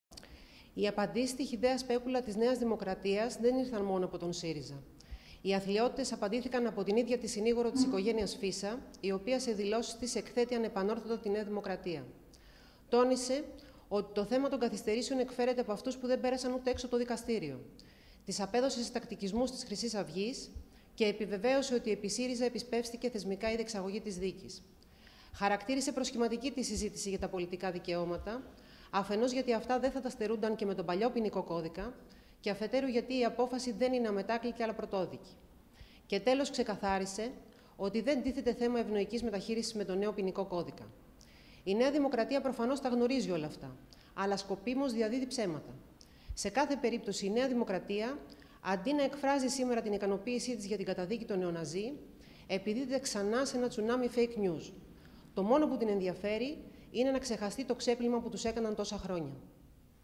Τ. Καραγιάννη: "Η δικηγόρος της οικογένειας Φύσσα απαντά στις αθλιότητες και εκθέτει τη ΝΔ"
Δήλωση (on camera) της αναπληρώτριας εκπροσώπου Τύπου του ΣΥΡΙΖΑ – Προοδευτική Συμμαχία, Τάνιας Καραγιάννη, για τα fake news της ΝΔ μετά την καταδίκη της Χρυσής Αυγής